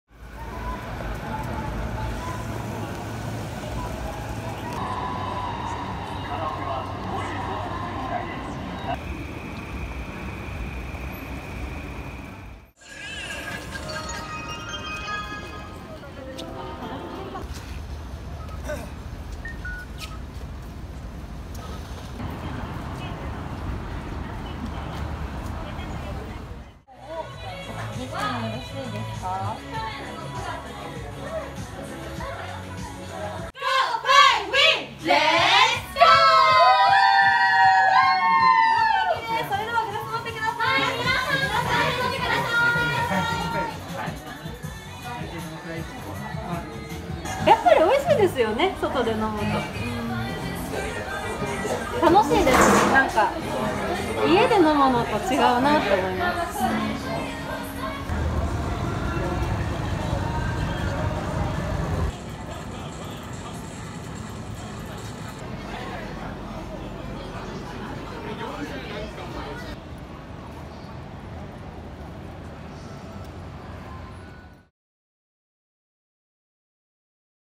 宣言解除の夜の街 戻りつつある人波
全国で緊急事態宣言が解除された。夕暮れの東京都内は、帰宅を急ぐ人が増え、夜の繁華街にも人波が戻りつつあった。

※日本経済新聞社の動画について改ざんや、許可なく商用・営利 目的で利用することを禁じます。チア居酒屋　繁華街